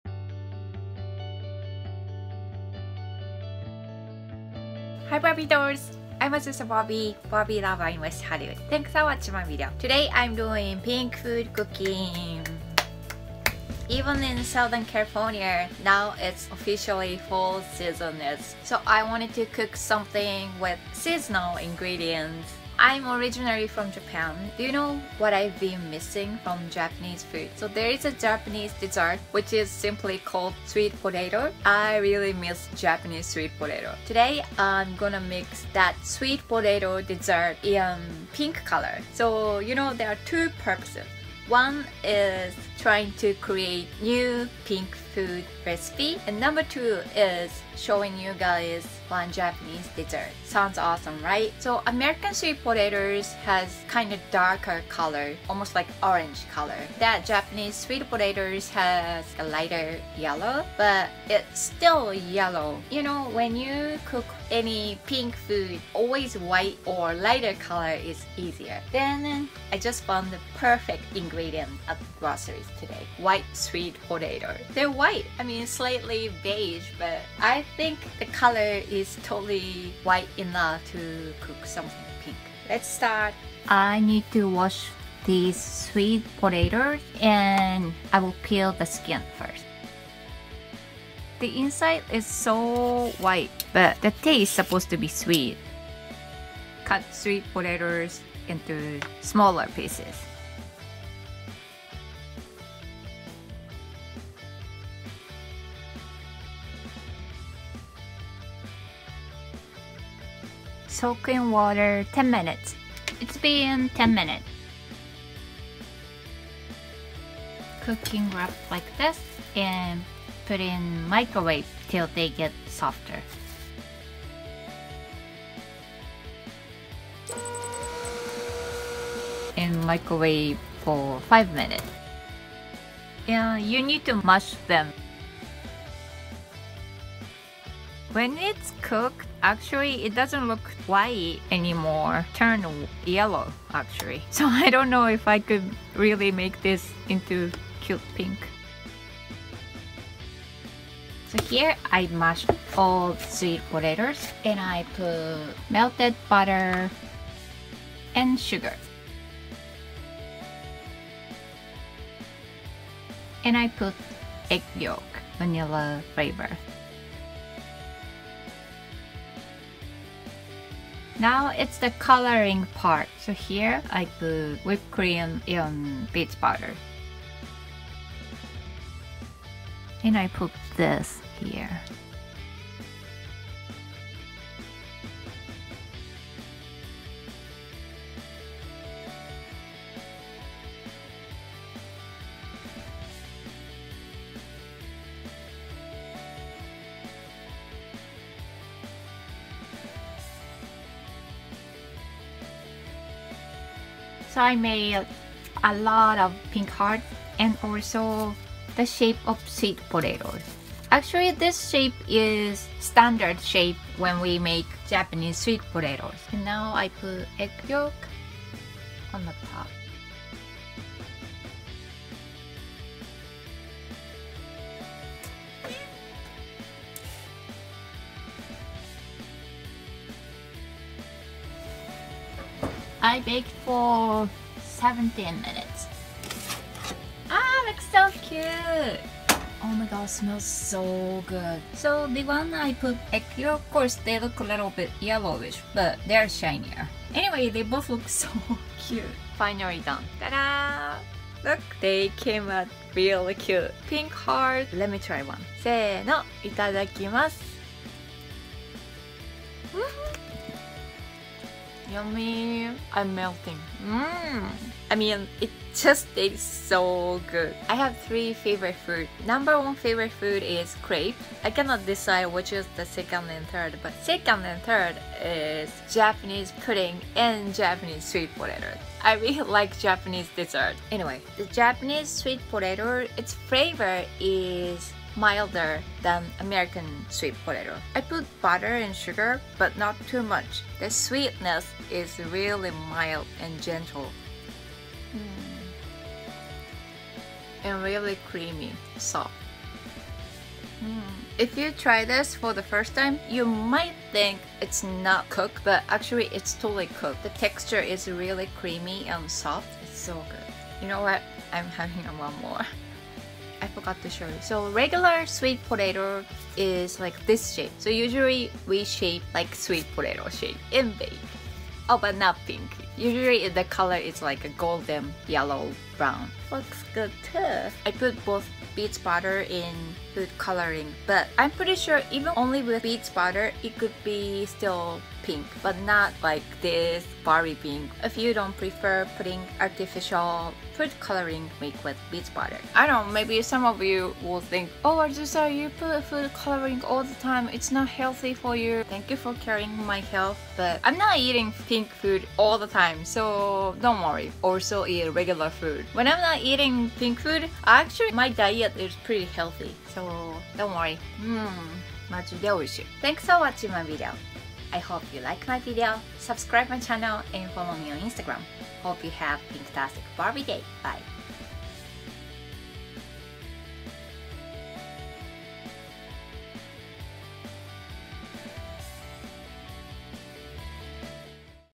[COOKING] PINK HEART♡ Japanese Sweet Potato Dessert♡ AZUSA BARBIE
I'm BARBIE LOVER in West Hollywood, California!
Sharing my collections and my Barbie life!!

Part of profit from views on my channel will be donated to Mattel Children's Hospital♡♡♡